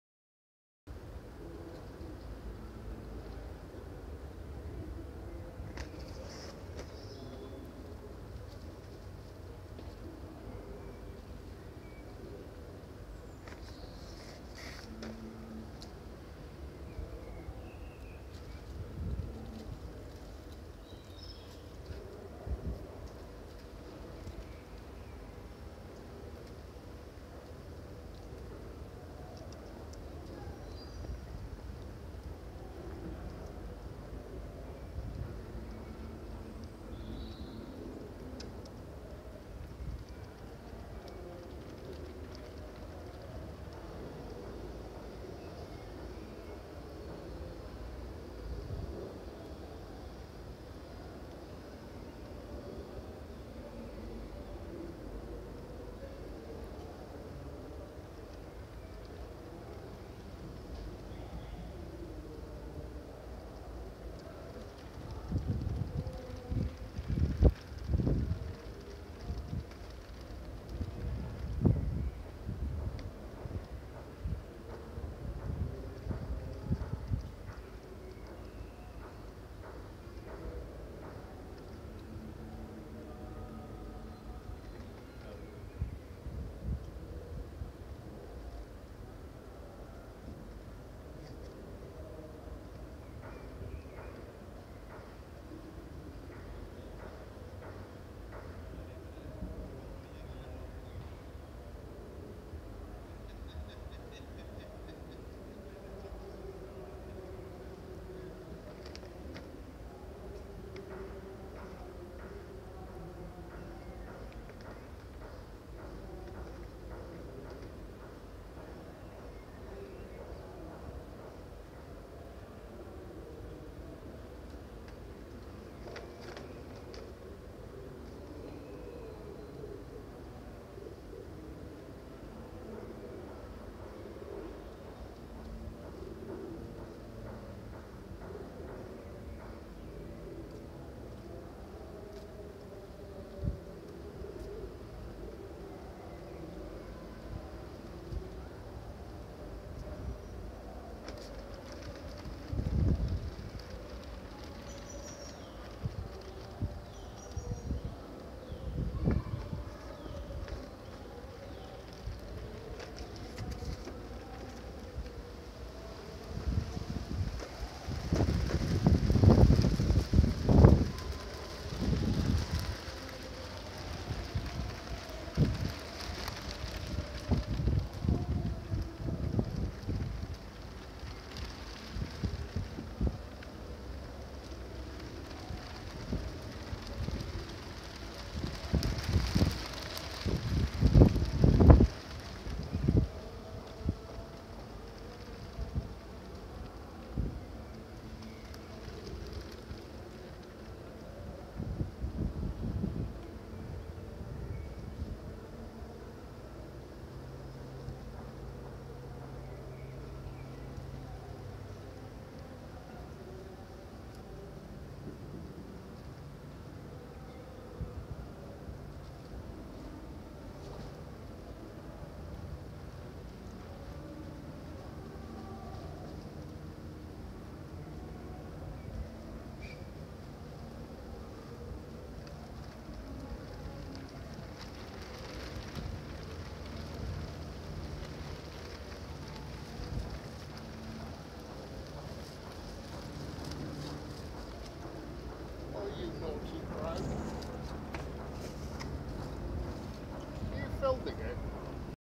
A crow rummaging for food
in the gardens around St Paul's Church in Covent Garden